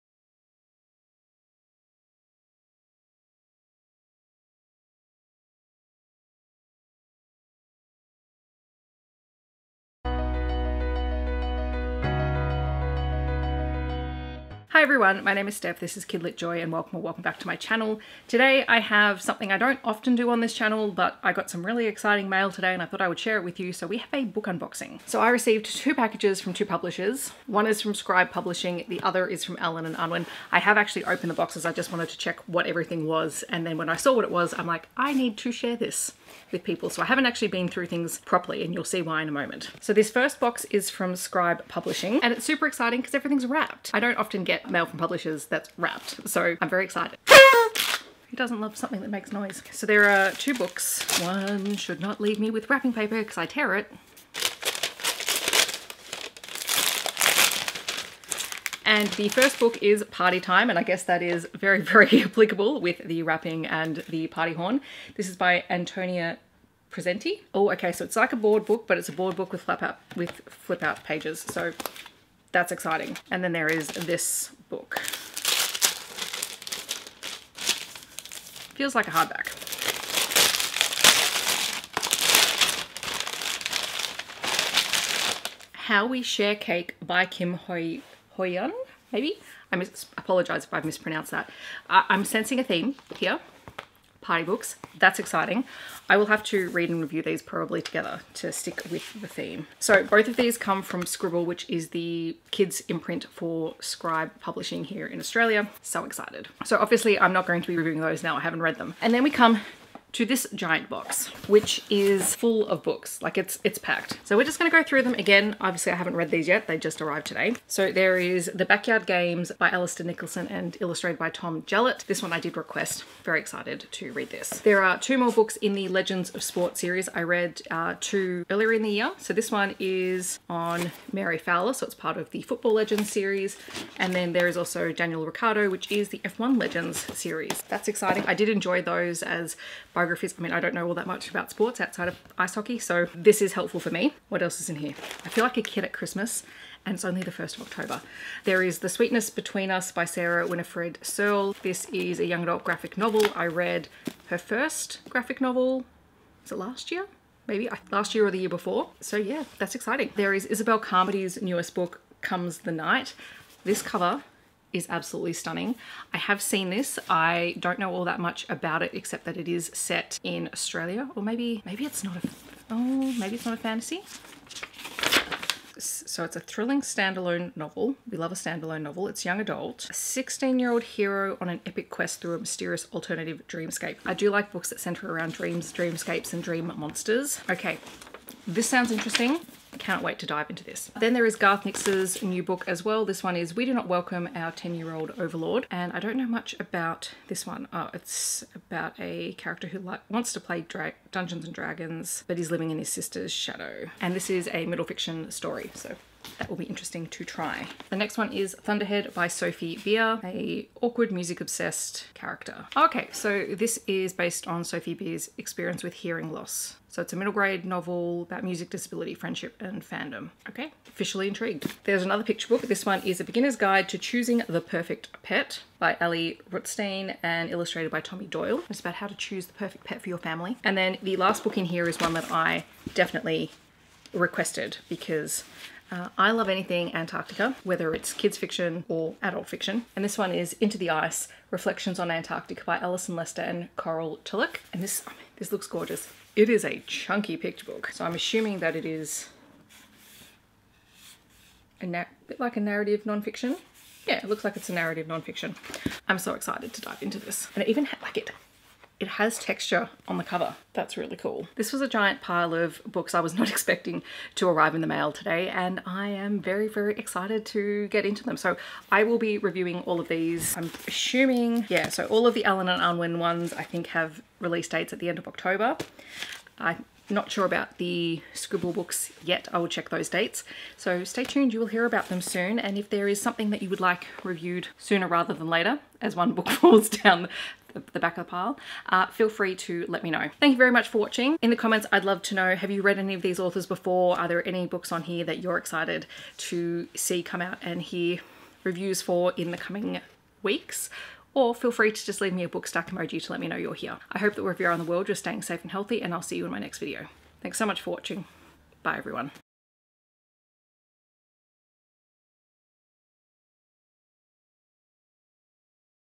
Book Haul Unboxing
This week I received some exciting book mail from publishers, so here’s an impromptu book haul & unboxing!

Thank you to the publishers @allenandunwin and @Scribble Kids Books for the review copies. All thoughts and opinions are my own.

BOOKS MENTIONED
Party Rhyme - Antonia Pesenti
How We Share Cake - Kim Hyo-eun
The Backyard Games - Alister Nicholson
Football Legends: Mary Fowler - Kit Cross & Leigh Hedstrom
F1 Legends: Daniel Ricciardo - Kit Cross & Leigh Hedstrom
The Sweetness Between Us - Sarah Winifred Searle
Comes the Night - Isobelle Carmody
We Do Not Welcome Our Ten Year Old Overlord - Garth Nix
Thunderhead - Sophie Beer
A Beginner’s Guide to Choosing the Perfect Pet - Ali Rutstein
Into the Ice - Alison Lester

TIMESTAMP:
0:00 Acknowledgement of Country
0:16 Intro
0:39 Books from Scribble
1:48 Books from Allen and Unwin
4:21 Wrap-Up